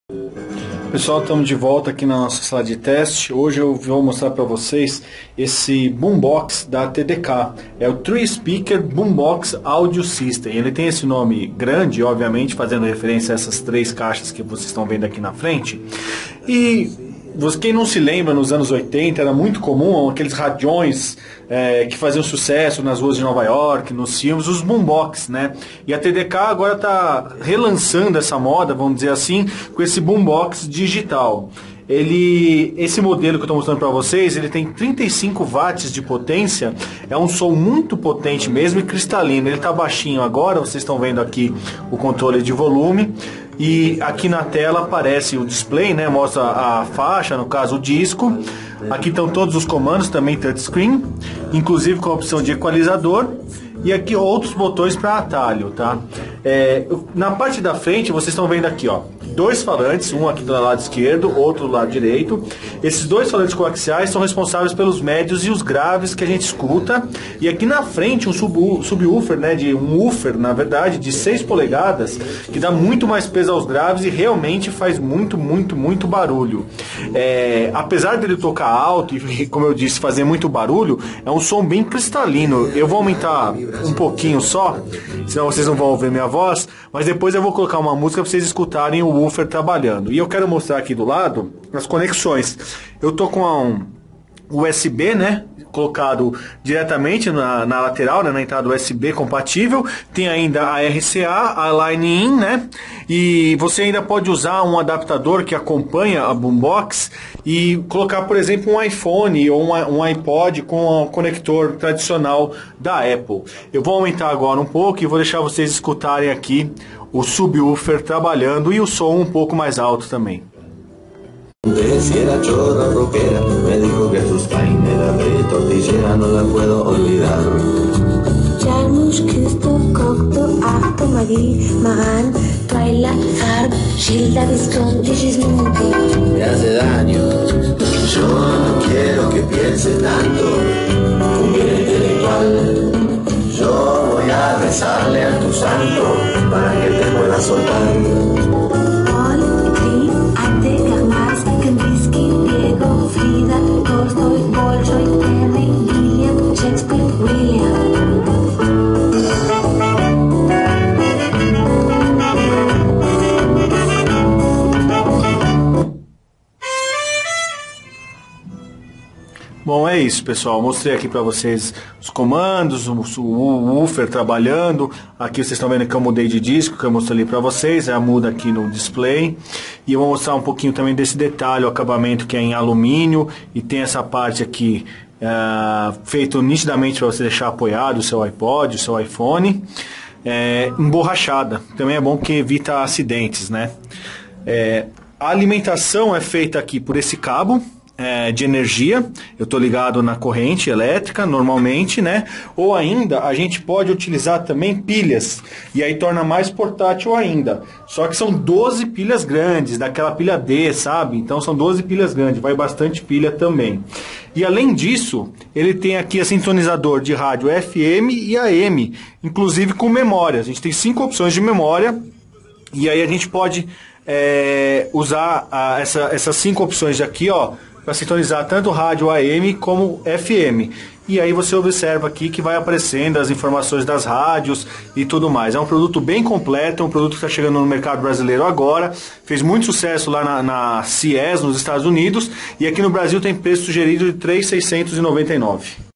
Conheça o 3 Speaker Boombox Audio System da TDK
A TDK resolveu revisitar a época lançando um Boombox digital, o 3 Speaker Boombox Audio System, que você pode ver no vídeo acima.
Ele tem um som impressionante, com 35W de potência, soando de maneira nítida, sem distorções e com boa leitura de todas as frequências.
Na parte da frente, dois alto-falantes coaxiais, responsáveis pelos sons médios, graves e agudos e na parte central um woofer de 6 polegadas para dar mais peso aos graves.
A alimentação é feita com 12 pilhas 'D' ou cabo de força AC.
Na lateral as entradas RCA, USB, Line in e ela ainda traz um cabo de 30 pinos para iPod/iPhone. Dá até para usar a Boombox com outros instrumentos, como guitarras, teclados e microfones.
O 3 Speaker tem ainda sintonizador de Rádio AM/FM, com 5 opções de memória, e todos os comandos são feitos no painel retroiluminado sensível ao toque. O produto tem preço sugerido de R$ 3.699.